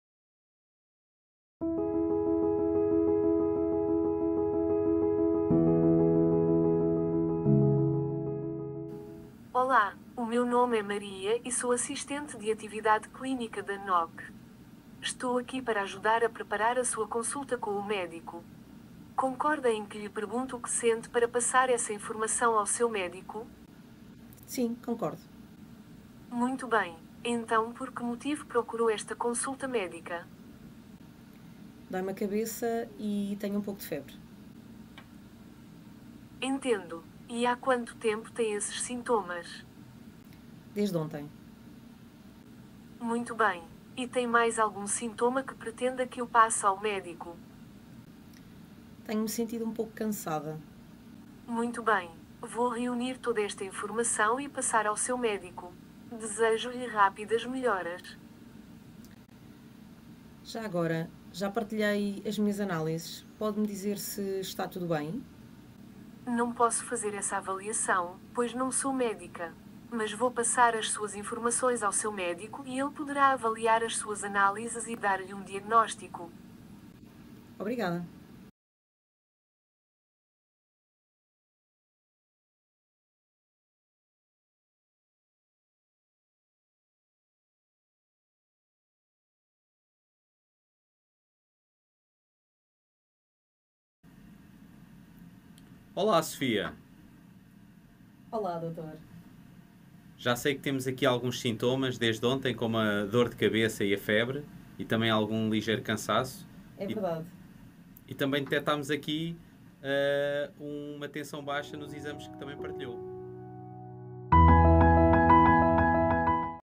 LLM-powered clinical practice assistant AVATAR | knok & Google | Premiere @ IDC Directions Sep 2023
The digital health company knok and Google announced the development of an LLM-Powered clinical practice assistant that engages in natural conversations with patients to collect symptoms, summarise them and  automatically pass them on to their personal EHR. The doctor then has prompt access to the symptoms and data collected by Maria prior to the video consultation.

The avatar was premiered during the Google Cloud pitch at the IDC Directions 2023 event in Lisbon, Portugal.

LLM-powered avatar: jointly developed by knok and Google.